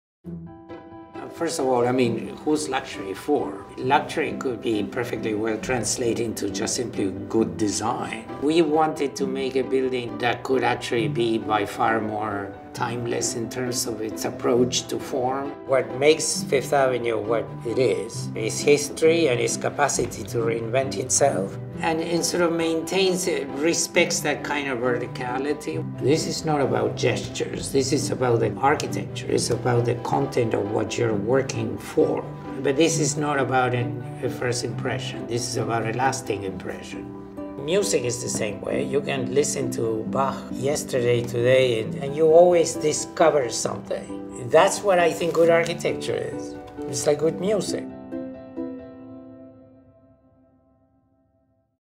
277 Fifth Avenue by Rafael Viñoly
Rafael Viñoly’s vision for 277 Fifth Avenue leaves a lasting impression on an iconic New York streetscape. His contemporary design features elegant proportions and corner exposures through floor-to-ceiling windows from every residence and a façade composed of high-performance cast panels, custom fabricated in Finland in a bespoke Indigo color.

A standout feature is a quartet of distinguished loggias—sheltered, double-height, open-air spaces—for leisure and entertaining. The double-height, open-air loggias spiral downward from the top of the tower and are seemingly carved out of the building’s corner. Situated on the tower’s uppermost floors are three penthouses—each with four-bedrooms; ceiling heights in excess of 13 feet; and expansive living, dining, and family rooms with multiple exposures. Source by 277fifth. Info project